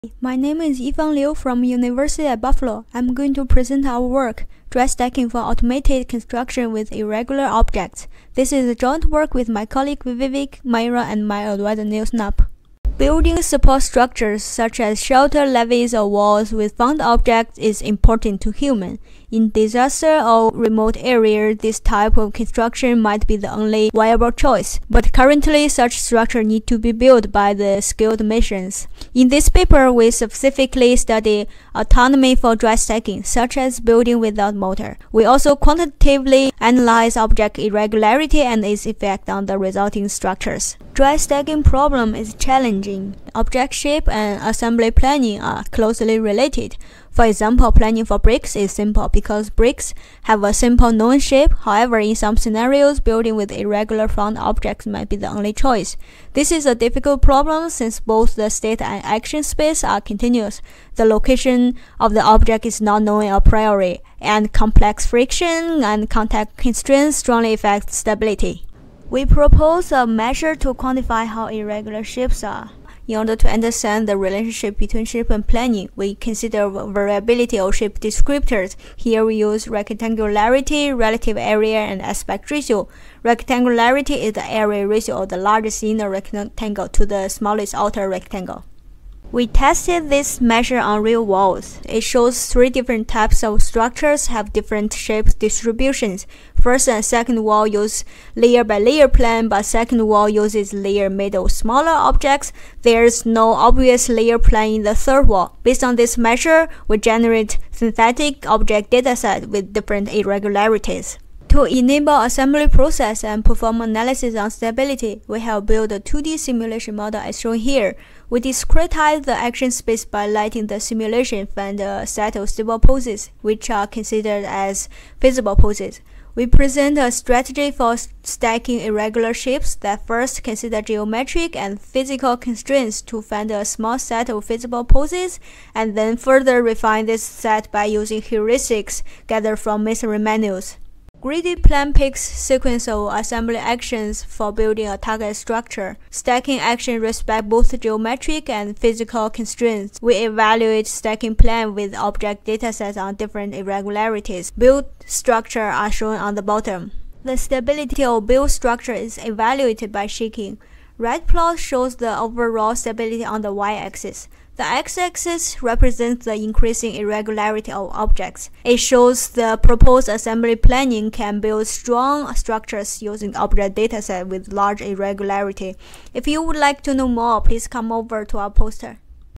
Dry Stacking for Automated Construction with Irregular Objects
Interactive Session Wed PM Pod N.8
Authors: Thangavelu, Vivekanandhan; Liu, Yifang; Saboia Da Silva, Maira; Napp, Nils
Title: Dry Stacking for Automated Construction with Irregular Objects

Abstract:
We describe a method for automatically building structures from stacked, irregularly shaped objects. This is a simplified model for the problem of building dry-stacked structures (i.e. no mortar) from found stones. Although automating such construction methods would be ideally suited for disaster areas or remote environments, currently such structures need to be built by skilled masons. No practical methods for automating the assembly planning process are known. The problem is challenging since each assembly action can be drawn from a continuous space poses for an object and several local geometric and physical considerations strongly affect the overall stability. We show that structures that are built following a stacking order for perfect bricks can accommodate a limited amount of irregularity, however, their performance degrades quickly when objects deviate from their ideal shape. We present a strategy for stacking irregular shapes that first considers geometric and physical constraints to find a small set of feasible actions and then further refines this set by using heuristics gathered from instructional literature for masons. The proposed method of choosing assembly actions allows construction with objects that contain a significant amount of variation.